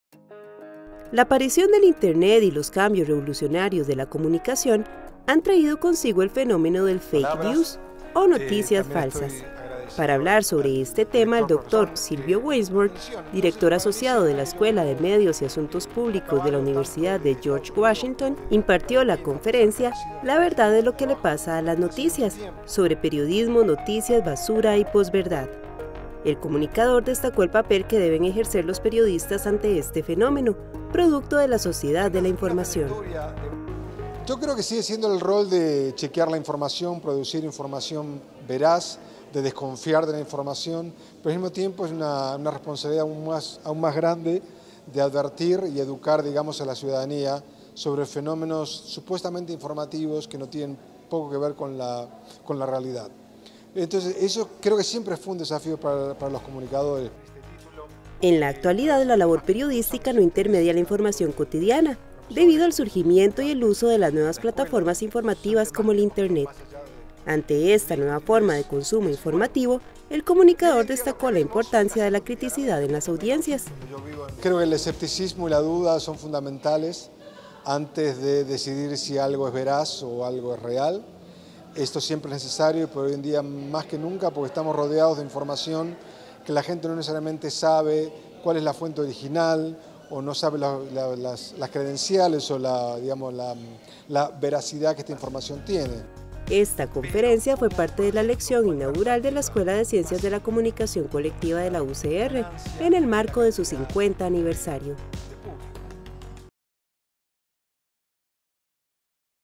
Lección Inaugural ECCC: Fake News y retos en la sociedad de la información
Silvio Waisbord, doctor en sociología de la Universidad de California, visitó la UCR para impartir la Lección Inaugural de la Escuela de Ciencias de la Comunicación Colectiva. En el contexto de las noticias "basura" o los Fakenews, el comunicador analizó los conceptos de posverdad y las nuevas realidades a las que se enfrentan los periodistas en la nueva sociedad de la información.